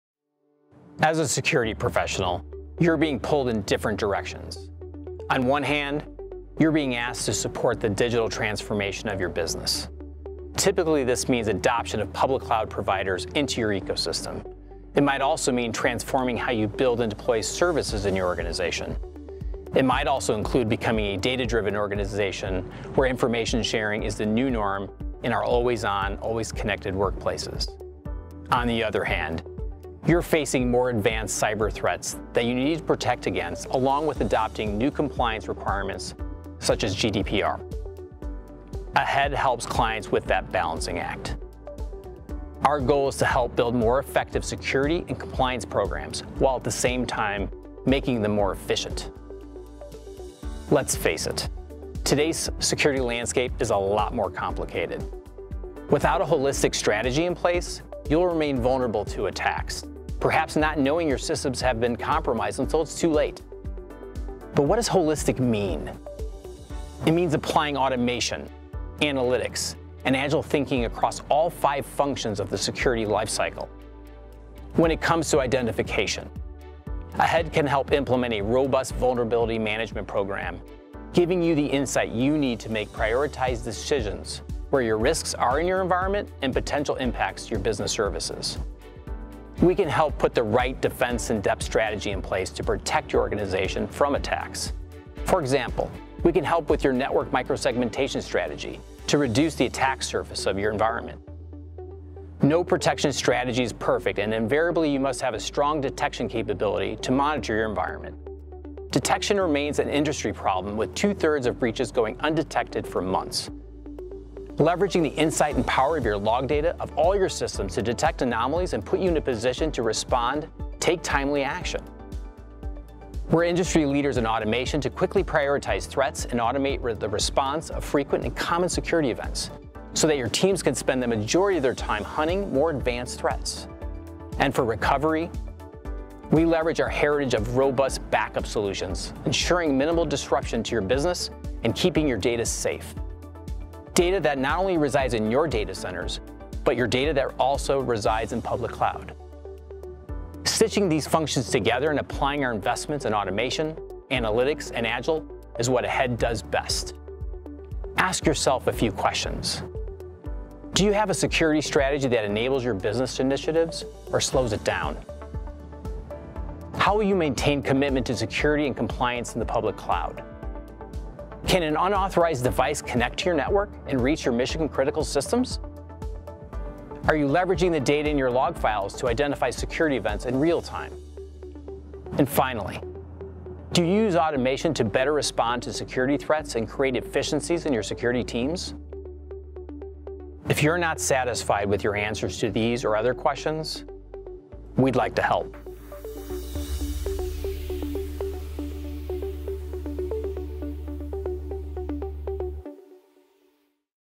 AHEAD Security Practice
As a security professional, you’re being pulled in different directions.

On the one hand, you are being asked to support the digital transformation of your business. On the other hand, you’re facing more advanced cyber threats that you need to protect against, along with adopting new compliance requirements.

AHEAD helps clients with that balancing act. Our goal in Security is to help build more effectivesecurity and compliance programs, while at the same time, making them more efficient.